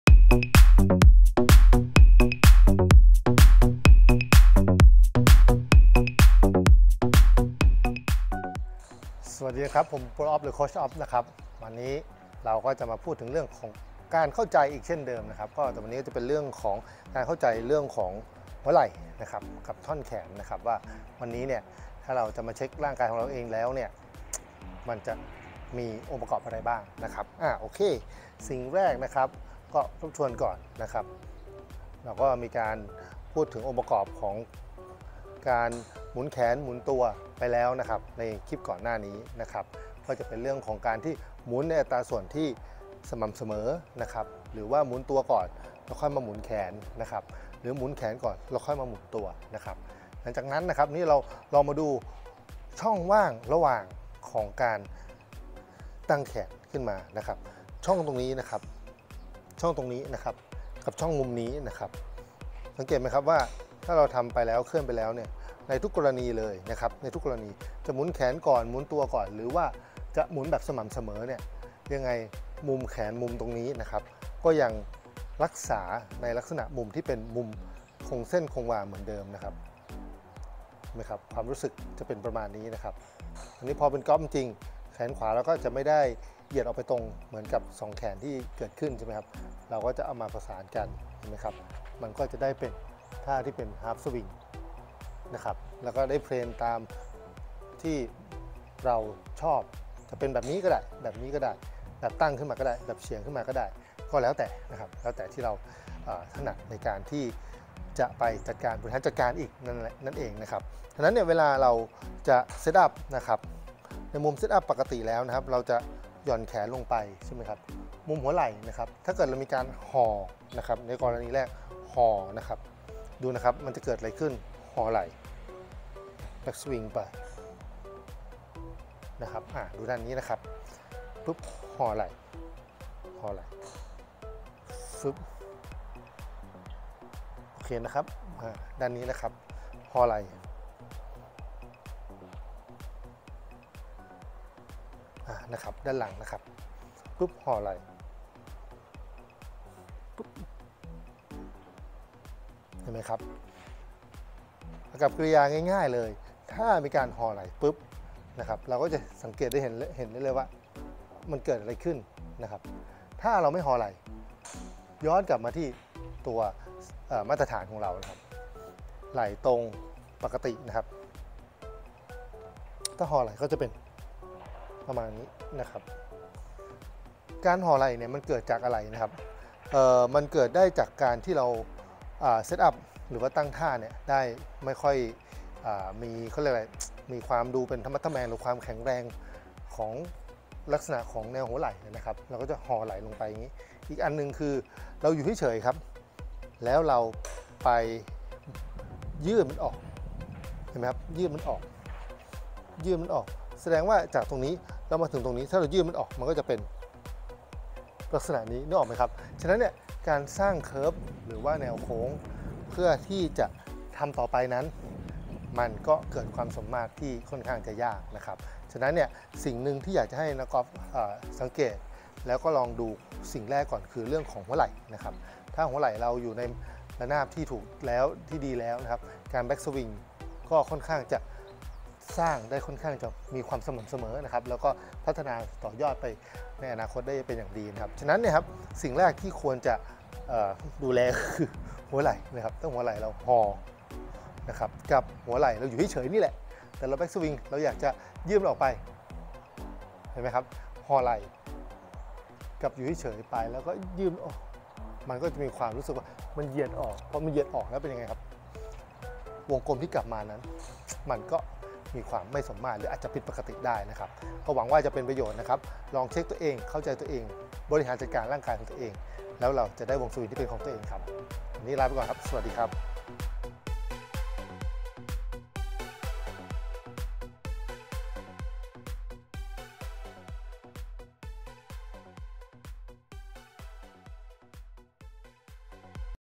การเข้าใจ - (4) - หัวไหล่ของเรา ในการสวิงกอล์ฟ
การห่อไหล่ เป็นส่ิงที่ควรทำความเข้าใจ เพราะโดยส่วนใหญ่แล้วแล้วนักกอล์ฟทั่วไปจะมีอาการนี้กันแทบทุกคน แต่เมื่อเราเข้าใจแล้ว เราก็จะสามารถบริหารจัดการและสร้างความเข้าใจในการเคลื่อนไหวของร่างกายเราตามความสามารถของร่างกายเราได้เป็นอย่างดี

“กอล์ฟไม่ได้มีแต่สวิง”

เป็นตอนต่อจากคลิปที่แล้ว (ดูได้จาก Link ด้านล่าง)

คลิปสาม การเข้าใจ ข้อเหวี่ยง หรือ Pendulum ในการสวิงกอล์ฟ

คลิปสอง การเข้าใจ การบริหารจัดการร่างกายของตัวเองในการสวิงกอล์ฟ

คลิปแรก การเข้าใจการเคลื่อนไหวร่างกายของตัวเองในการสวิงกอล์ฟ